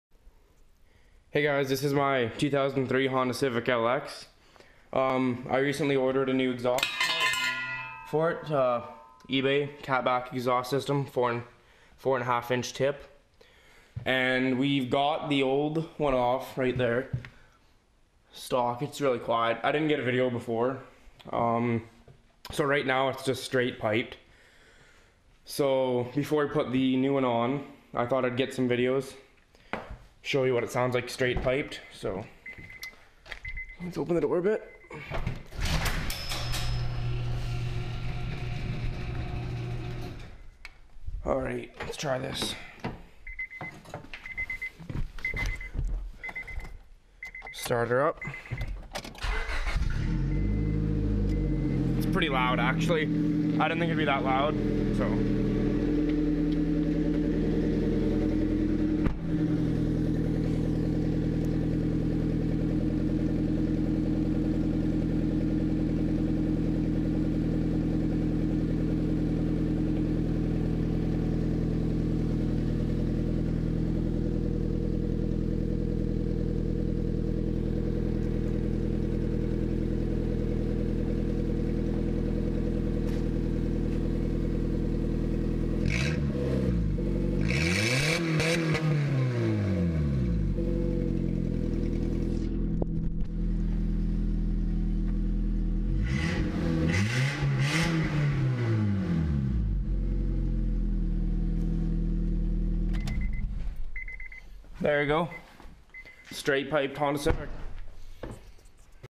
Honda Civic exhaust install part 1
putting my new ebay catback exhaust system on so took a quick video before it was on